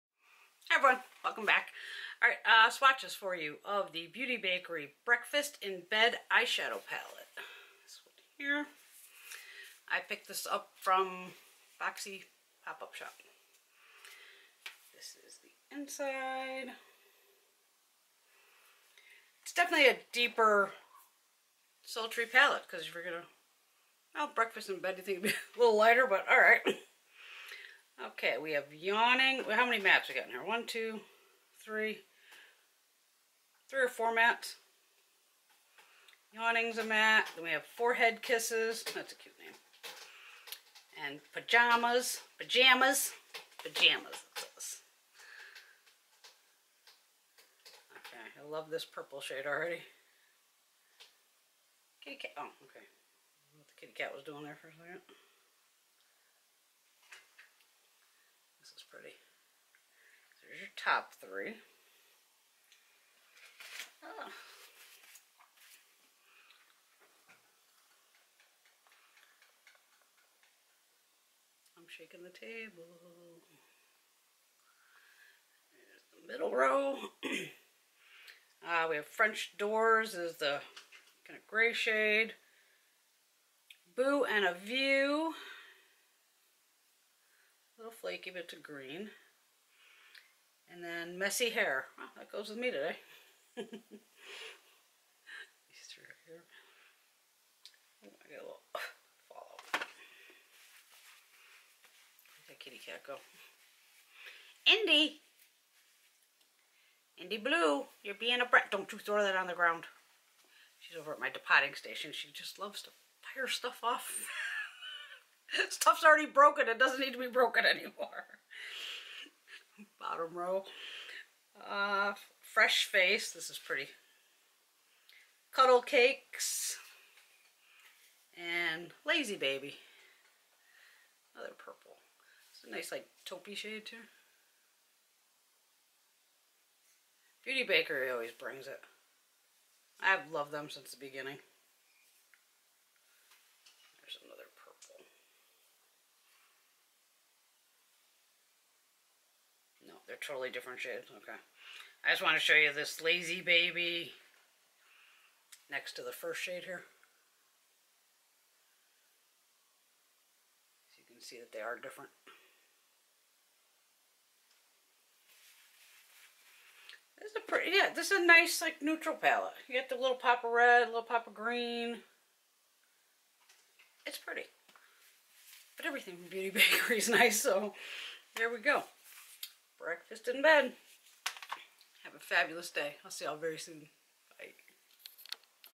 Swatches - Beauty Bakerie - Breakfast in Bed Eyeshadow Palette
Hi everyone. Here are swatches of the Breakfast in Bed Eyeshadow Palette from Beauty Bakerie. I picked this palette up from Boxy Charm Pop Up Shop. It is also available from Ulta Beauty & on the Beauty Bakerie Website.

also Macy's Beauty Box which is $15.00 per month.